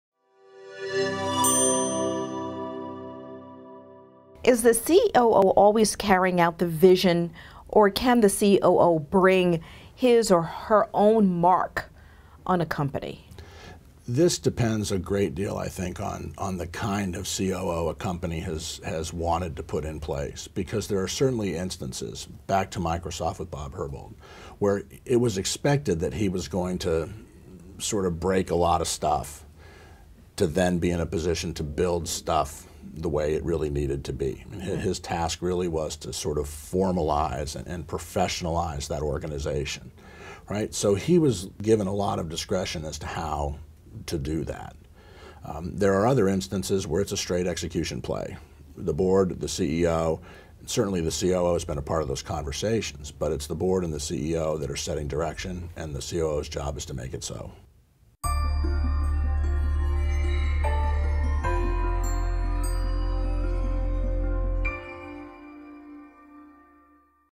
Can COOs institute some of their own ideas? By Nate Bennett
Nate Bennett, Ph.D., is an Associate Dean & Professor at the J. Mack Robinson College of Business at Georgia State University. Over the years, he has won numerous awards for his teaching. He is co-author (with Stephen Miles) of "Riding Shotgun: The Role of the COO" and is also the academic advisor to the COO Circle, a group Chief Operating Officers from the world's largest companies and most impactful non-profits. He is a frequent speaker and commentator in business press.

Nate Bennett talks about how the main job of the COO is to execute the strategy set out by the CEO. He provides some examples of COOs who were given the discretion to make policy in some areas on their own.